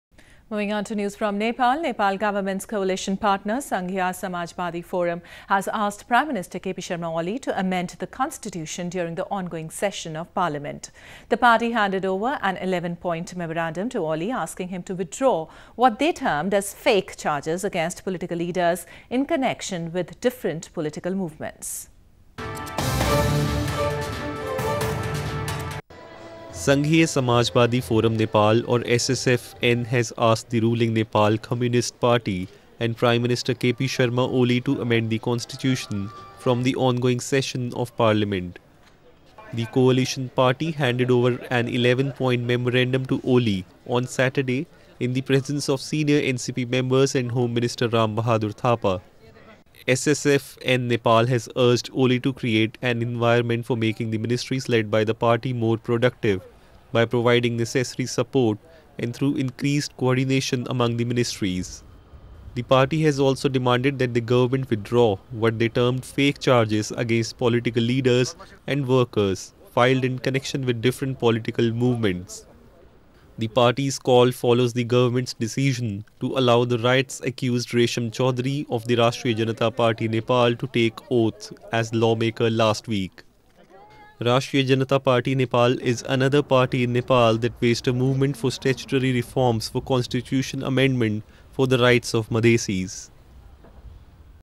Nepal News - Sanghiya Samajbadi Forum Nepal presses for constitution amendment
Nepal government's coalition partner, Sanghiya Samajbadi Forum has asked Prime Minister K.P Sharma Oli to amend the constitution during the ongoing session of Parliament. The party handed over an 11-point memorandum to Oli asking him to withdraw what they termed as “fake” charges against political leaders in connection with different political movements.